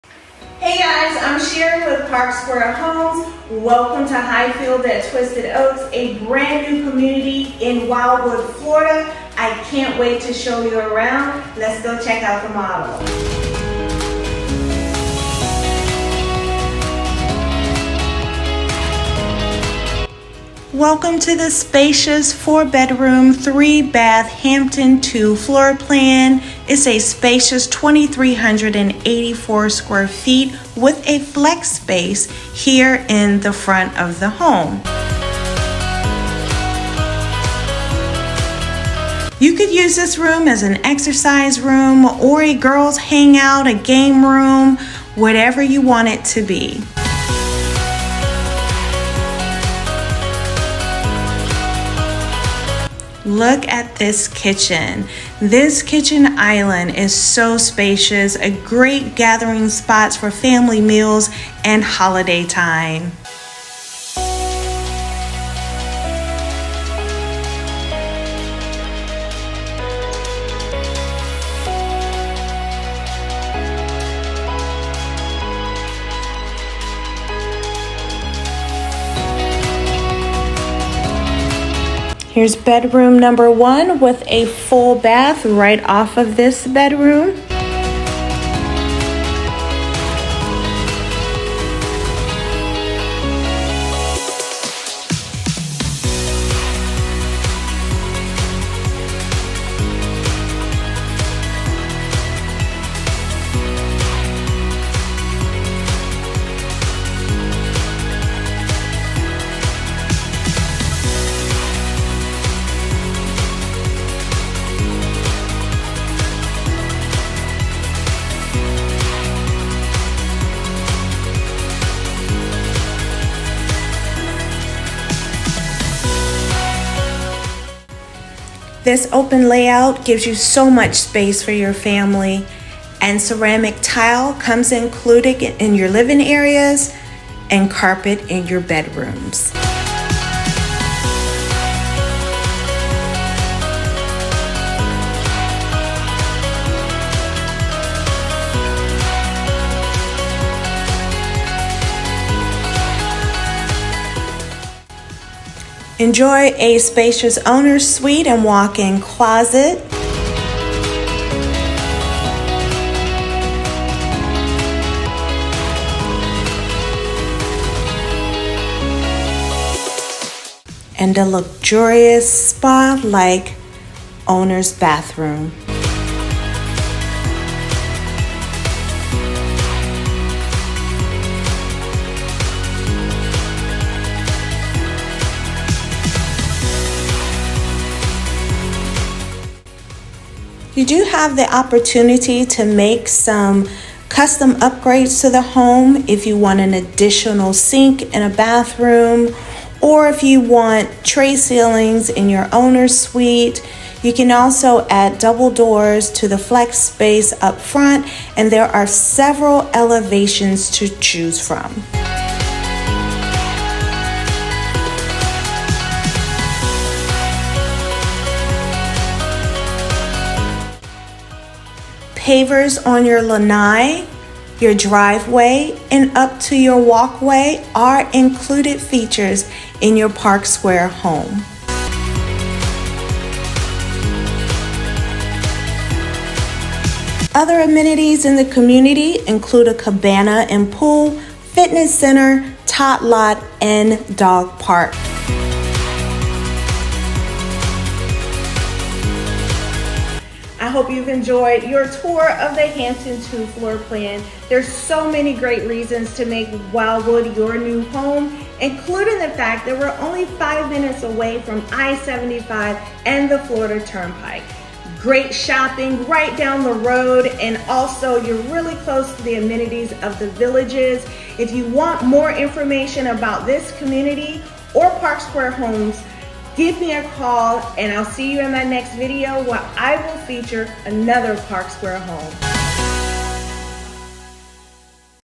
Twisted Oaks - 5 minutes away from The Villages
New Home Community in Wildwood, Florida
Park Square Homes
Hampton II floorplan - 2384 sq ft.
4 bedrooms, 3 baths + flex space

Sherri Ward
New Homes Counselor
sward@parksquarehomes